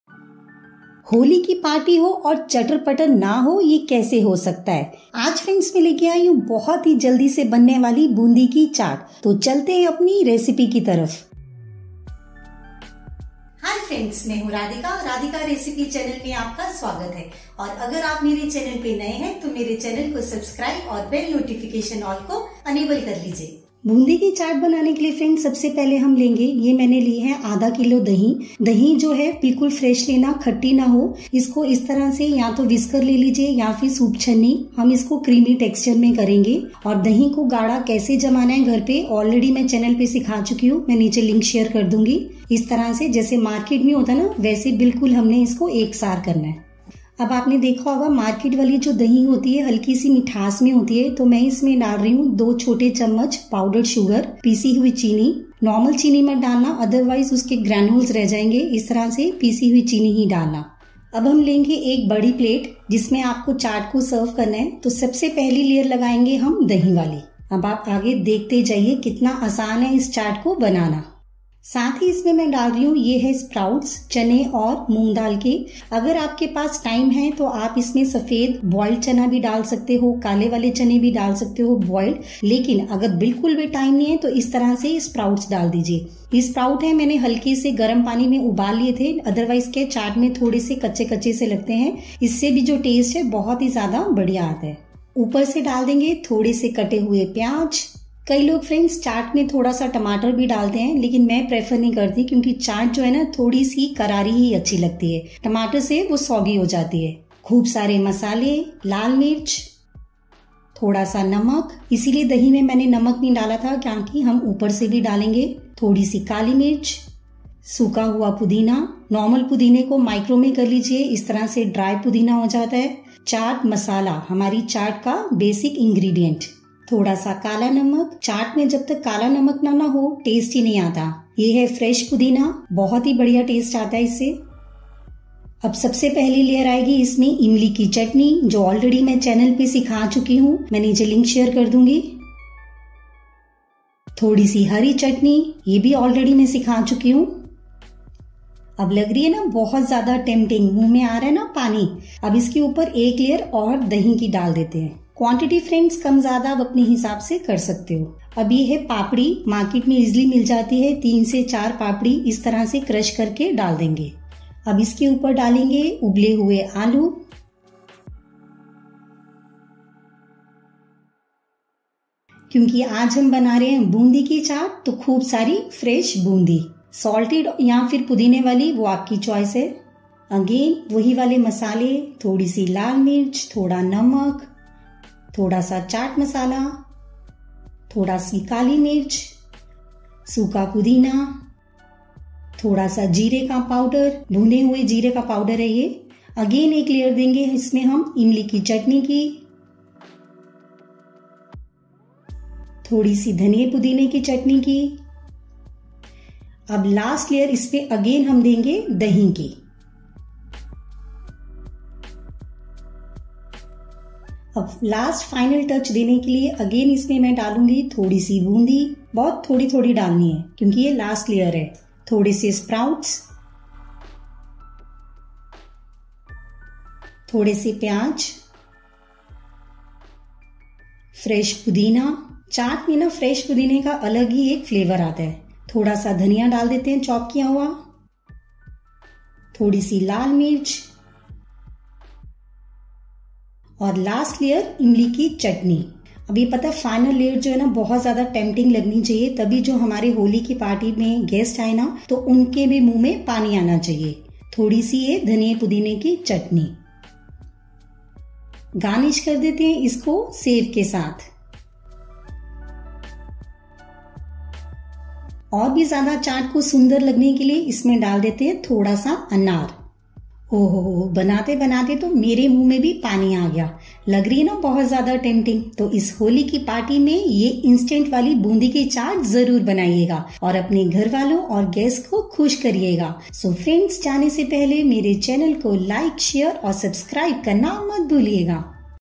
Chatpati Boondi Chaat Recipe 😋 😋 | Holi Special Chaat Recipe | Radhika recipes
Ingredients :-
Curd -500 gm
Powder sugar -2 tsp
Papdi -4/5
Sprouts ( chana / dal) -4 tbsp
Red mirch as per taste
Salt as per taste
Black salt as per taste
Black pepper -1/4 tsp
Dry mint powder as per taste
Jeera power -3 pinch
Chaat masala as per taste
Boondi handful
Imli chutney
Green coriander chutney
Sev for garnish
Pomegranate for garnish